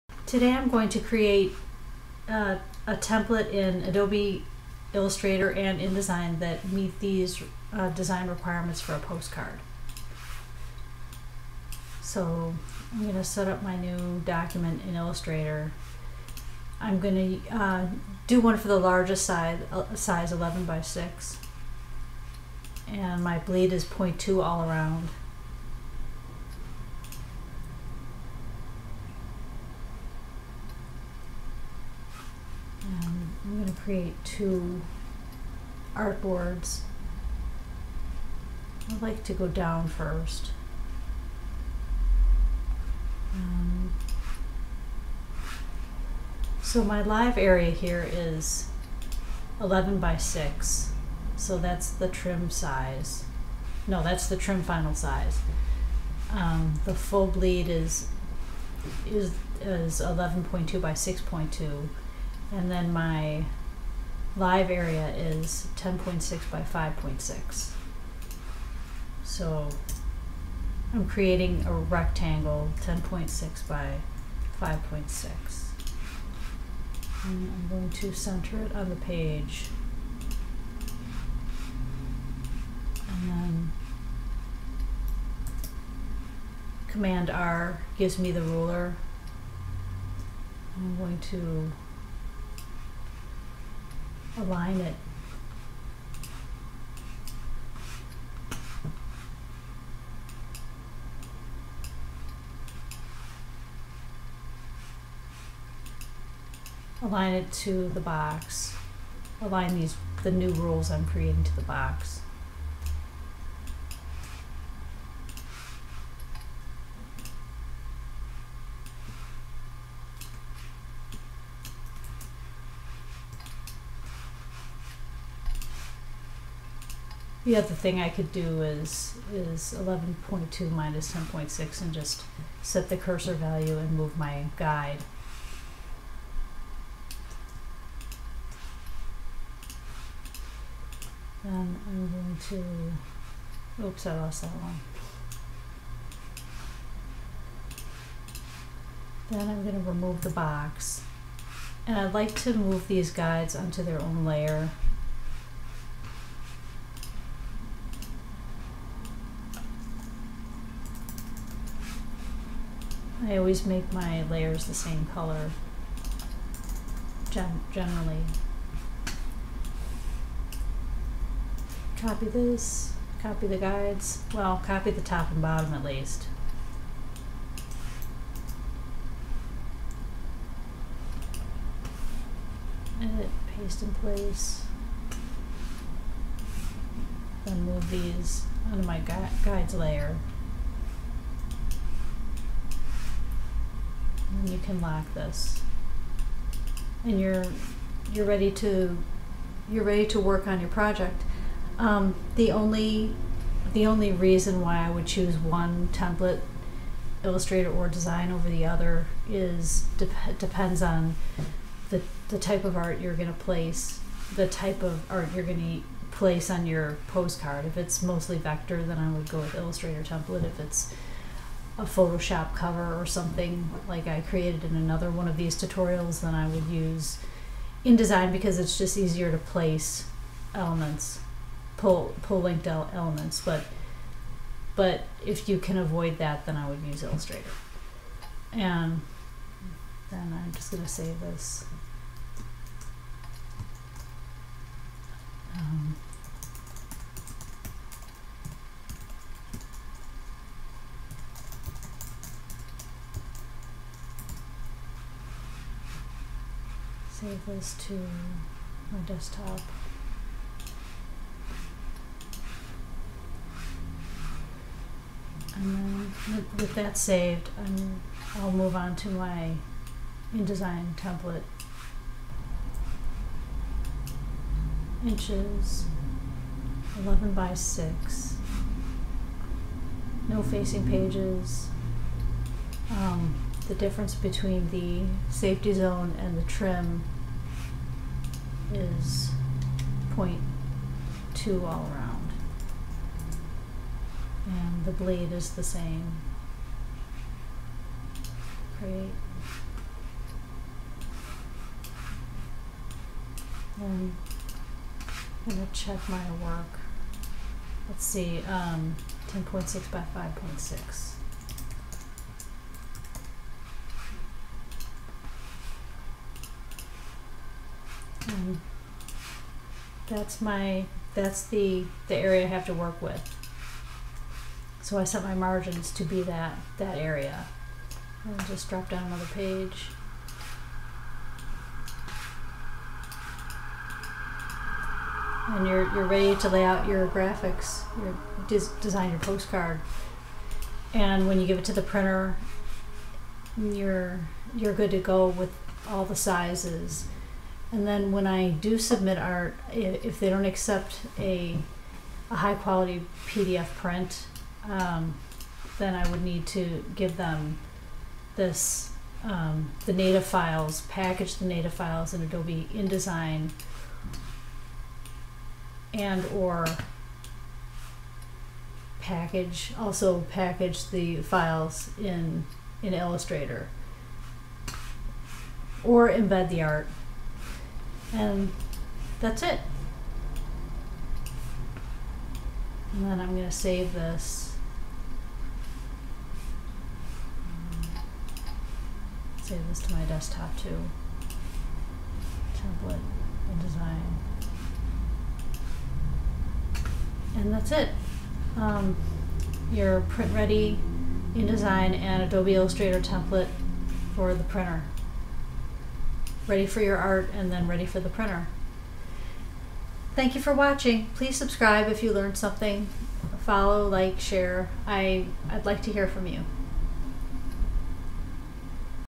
How to create a postcard template in Adobe® Illustrator® and Adobe® InDesign® with FREE DOWNLOAD
Here is an outline of the steps used in Adobe Illustrator:
1. Create your document (File, New)
2. Add your document dimensions (2 artboards)
3. Set your bleed dimension (background area that will be printed beyond the postcard trim size)
4. Select More Settings to control how the artboards will appear
5. Once the document is created, I align guides to a rectangle which matches the size of the live area and place the guides on a new layer above the art layer

Steps used in Adobe InDesign:
1. File, New Document (command + N)
2. Add the postcard length and width
3. Deselect Facing Pages checkbox
4. Set the margins to match the distance between the postcard trim edge and live area
5. Enter the bleed dimensions
6. Select Create
7. Drag a new page down from the Parent in the Pages panel for the back of your postcard